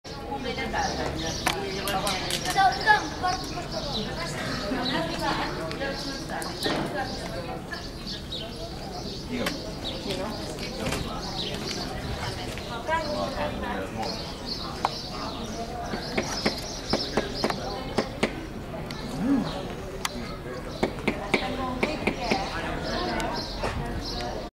Caminada-Pedalada Ruta de les colònies 2010. Vista a l´Ametlla de Merola
Caminada-Pedalada Ruta de les colònies 2010. Vista la plaça del teatre a l´Ametlla de Merola